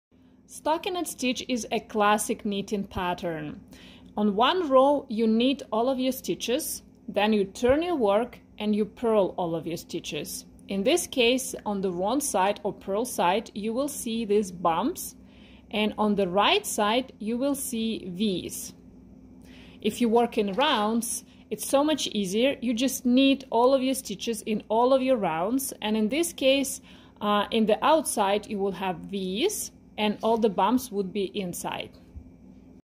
Stockinette Stitch pattern
Stockinette Stitch (ST st) - is a classic knitting pattern.

If you work flat:
r 1: knit all stitches
r 2: purl all stitches

If you work in rounds:
Knit all stitches in all rounds